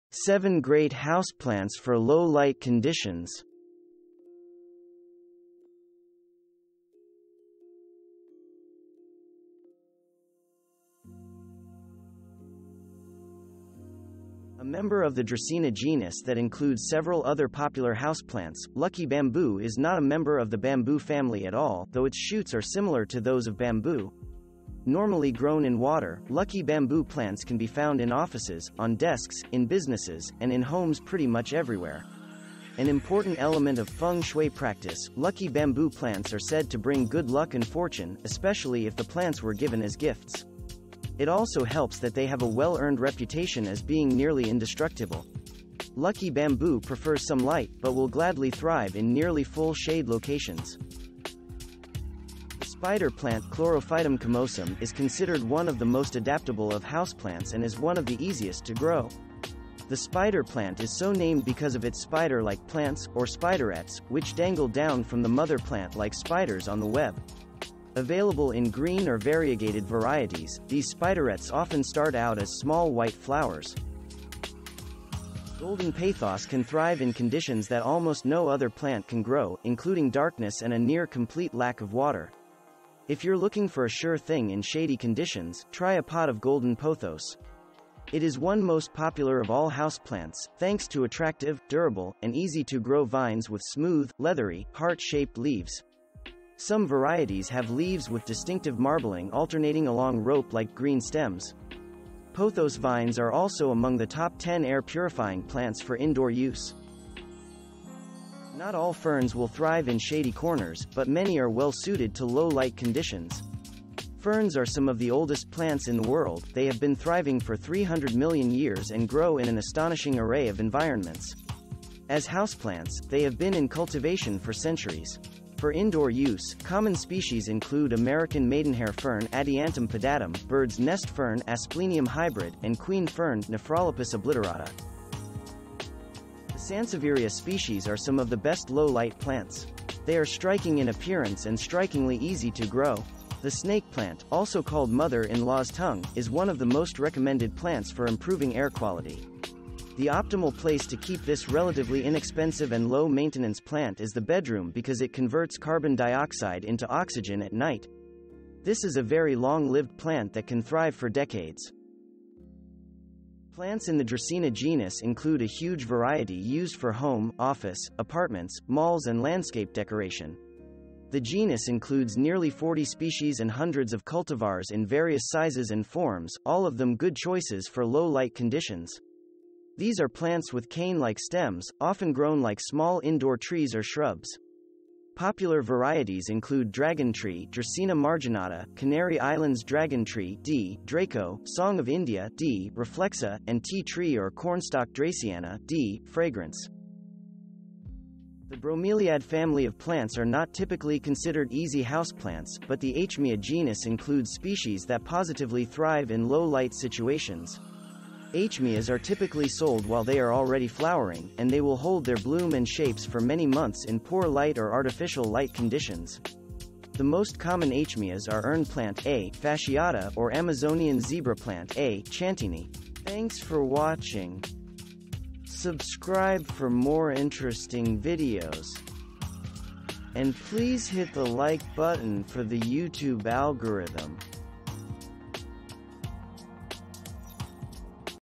7 Great Houseplants For Low-Light Conditions 🛋️
All plants need light, but some do surprisingly well in low-light indoor locations. Here are seven good plants for growing in shady indoor locations.
0:00 - Intro
0:00:12 -  Lucky Bamboo (Dracaena sanderiana)
0:00:21 -  Spider Plants (Chlorophytum comosum)
0:00:30 -  Golden Pothos (Epipremnum aureum)
0:00:39 -  Ferns (Several Genera)
0:00:48 -  Snake Plants (Sansevieria trifasciata)
0:00:57 -  Dracaena (Dracaena spp.)
0:01:06 -  Urn Plant or Zebra Plant (Aechmea asciata or A. chantinii)